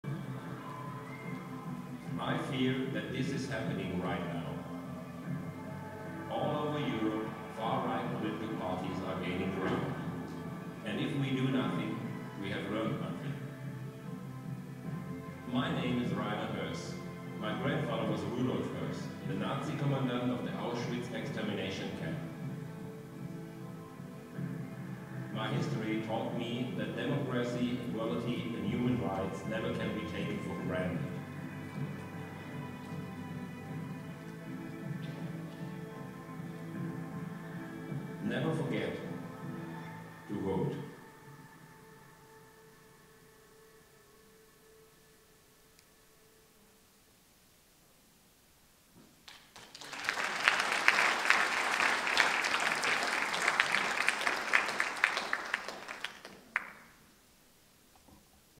Auschwitz-leader's kin, Rainer Hoess, appears in Swedish liberal party advert
Auschwitz-leader's kin, Rainer Hoess, appears in Swedish Social Democratic party's election advert.